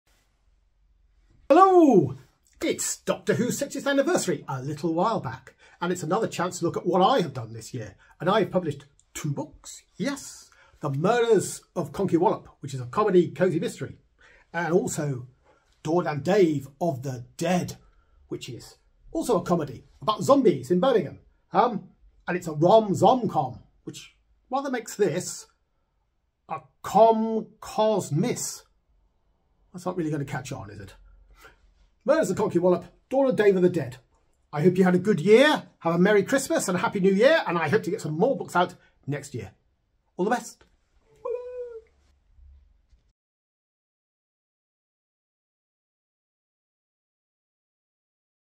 New Street Authors: 12 Authors for Christmas 10 : David Wake : Murders of Conky Whallop
New Street Authors: 12 Authors for Christmas
Number 10 : David Wake, Murders of Conky Whallop and Dawn of the Dead

For lots of indie authors with fascinating books for Christmas.
But do remember:  A book isn't just for Christmas, its for life!